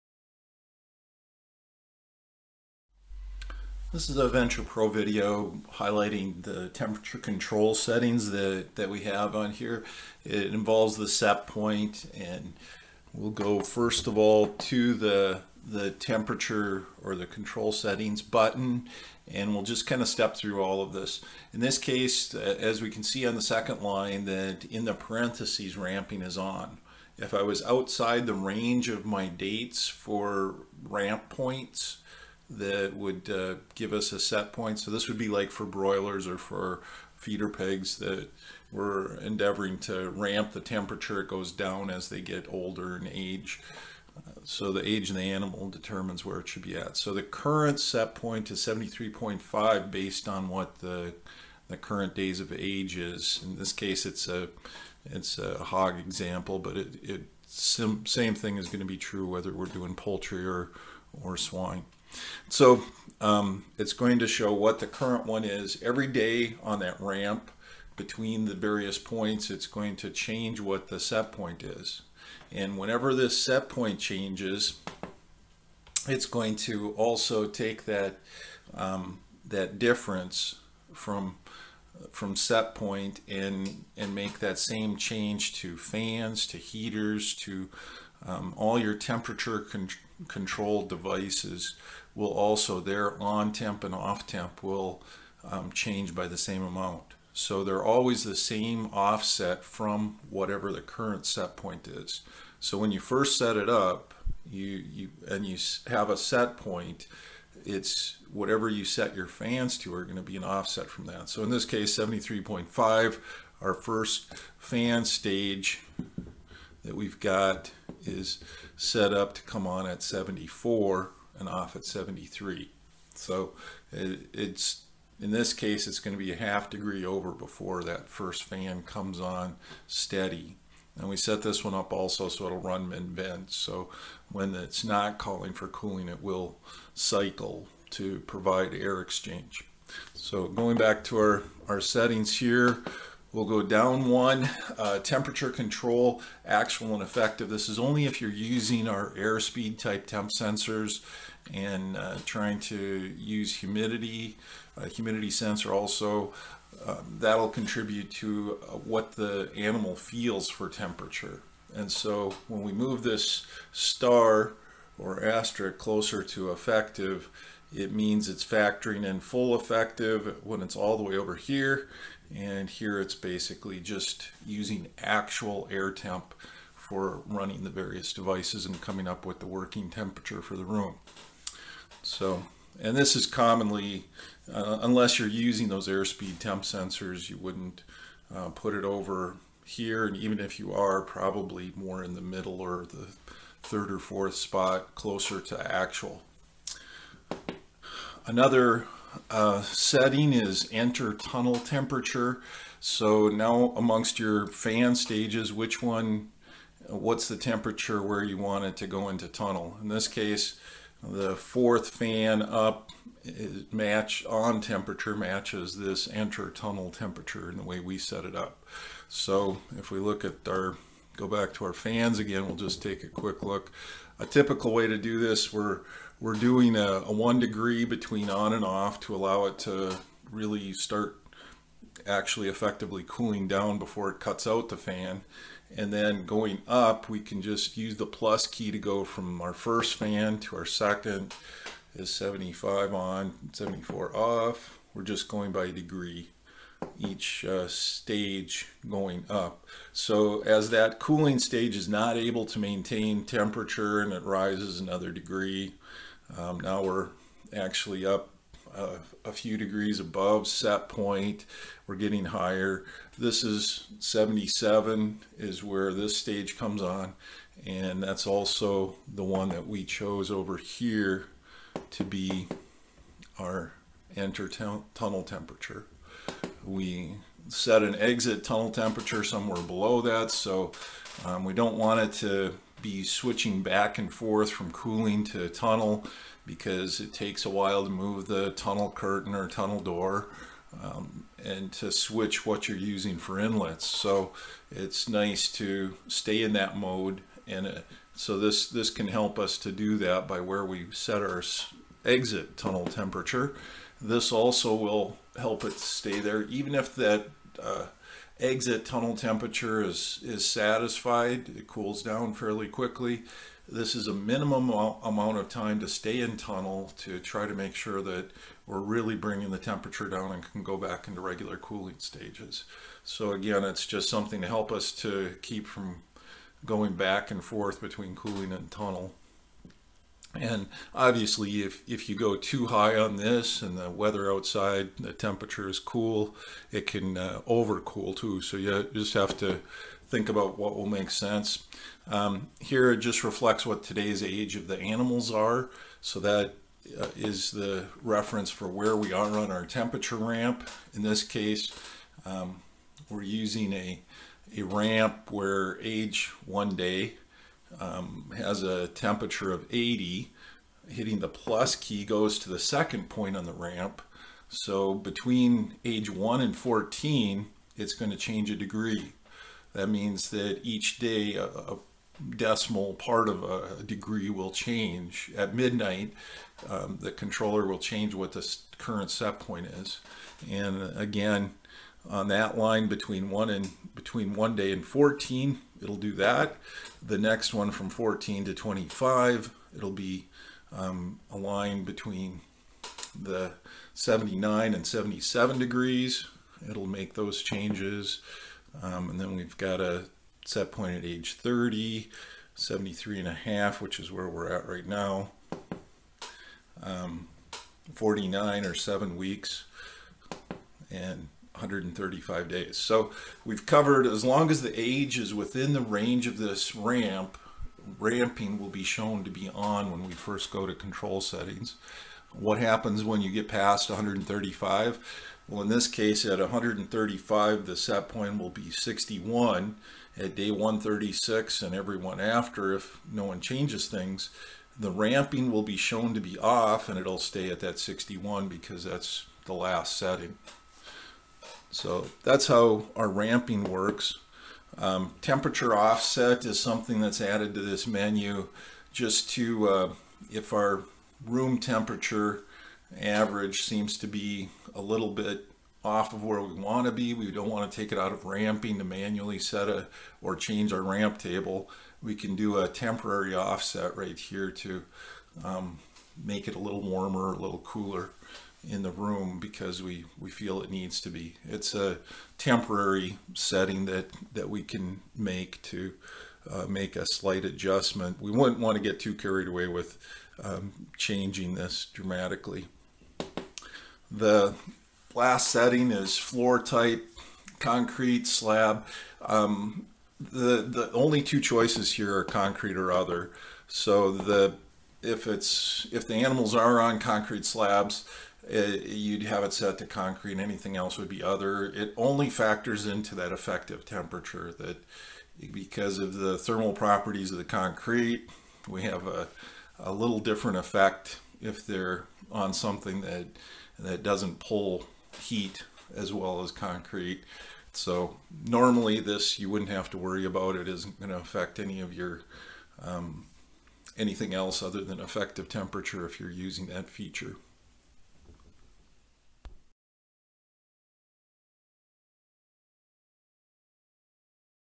Ventra Pro - Programming Control Settings
Setting the ramp temp points.  Age and temp for the desired ramp point.